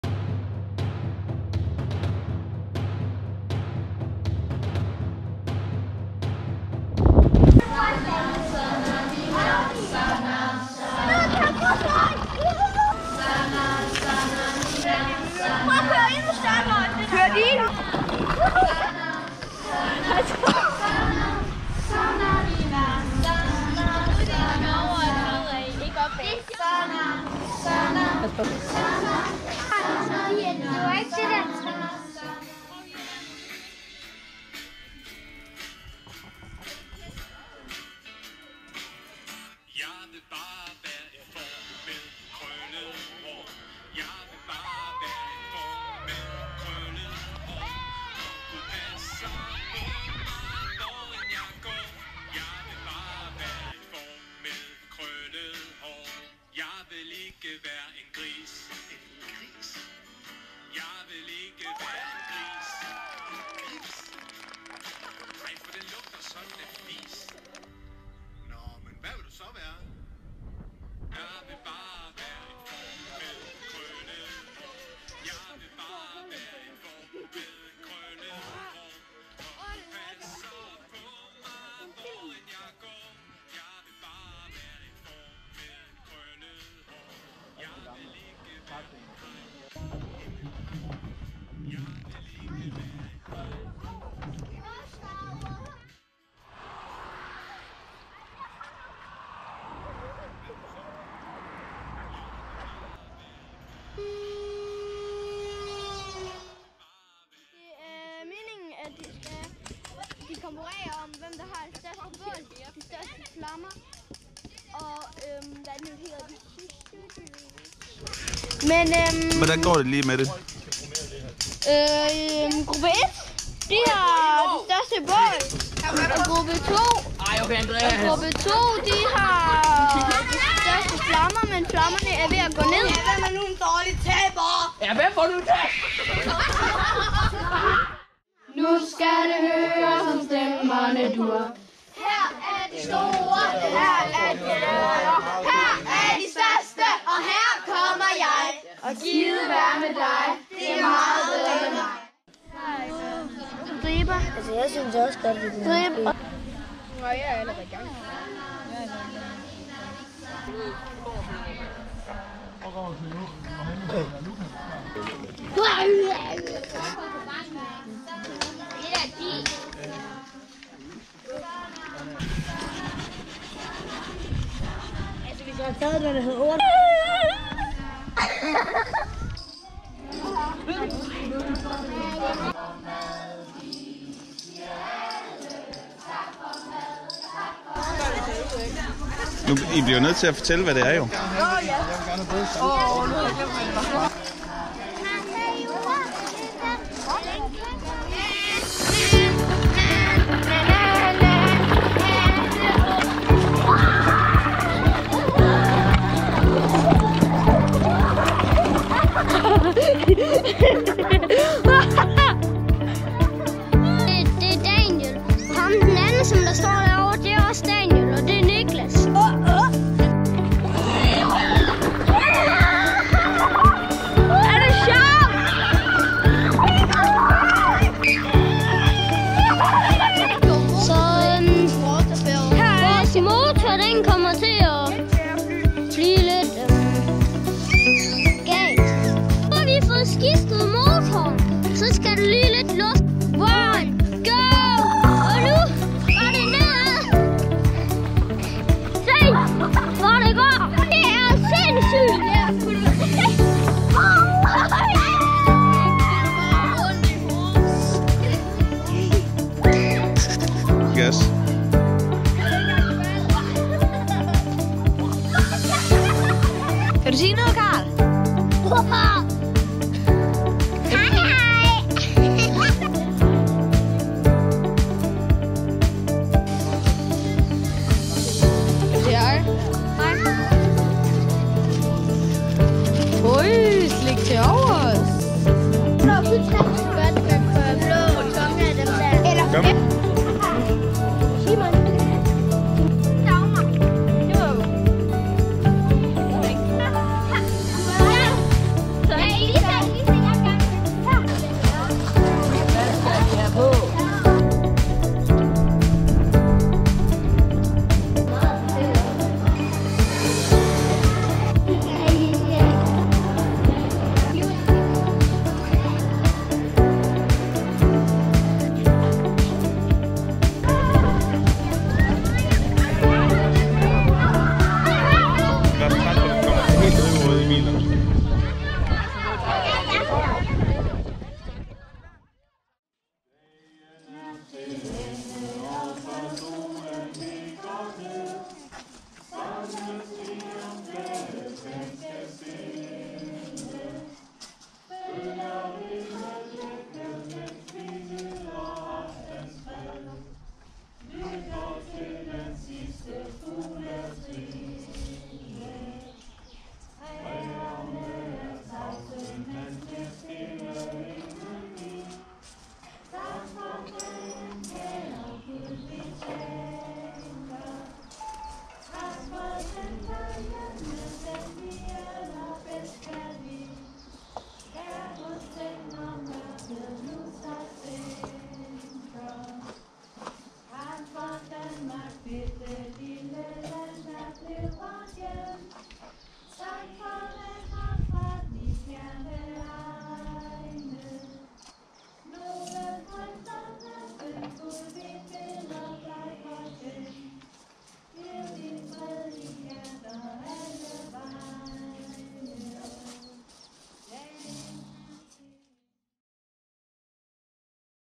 FDF Arnborg sommerlejr 2020
I år gik turen til Afrika, med Safari, Safaribiler, krybskytter og meget mere...